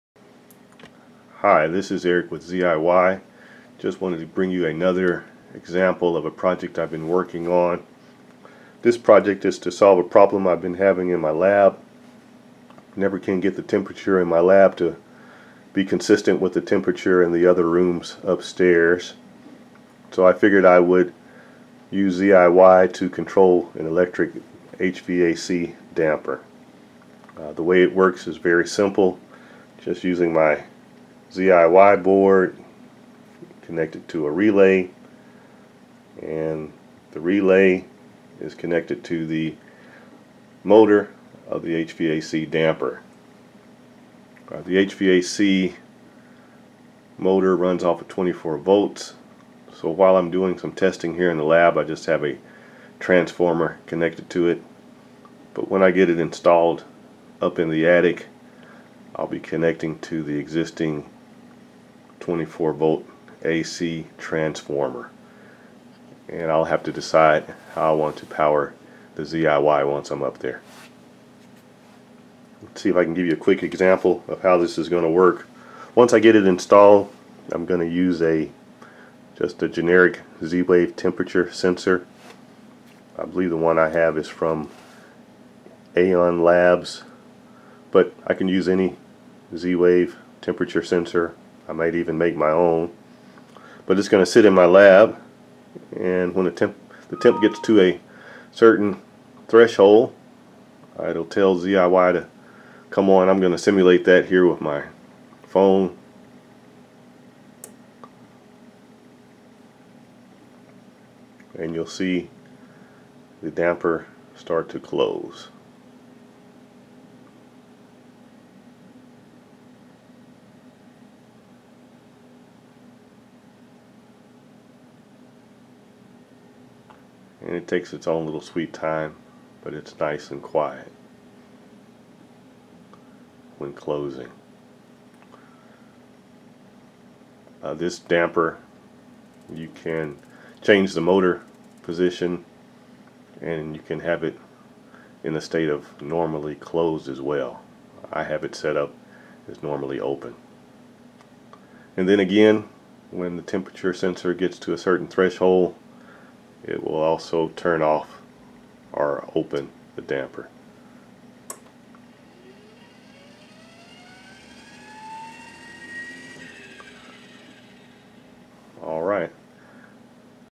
ZIY Automated HVAC Damper
ZIY is an Arduino compatible board with Z-Wave built-in. Build and connect your own devices to a smart home network.

community.ziy.io